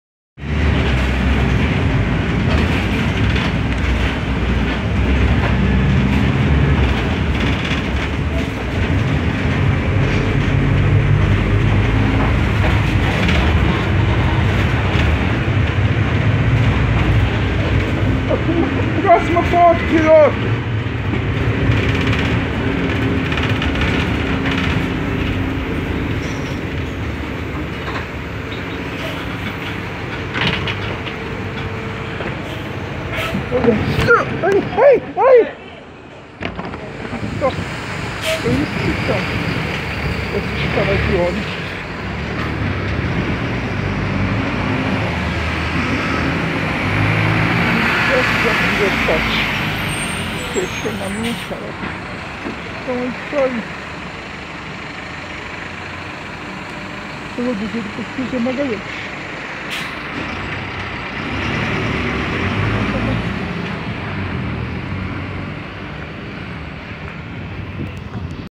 MOTORISTA IRRESPONSÁVEL FECHO A PORTA COMIGO DESCENDO E AINDA PAROU AFASTADO DO MEIO FIO
O motorista deste ônibus fechou a porta comigo descendo(poderia me machucar) e
ainda parou longe do meio fio me expondo ao risco dum atropelamento.
Infelizmente esses é o comportamento de alguns desses profissionais.

Namastê!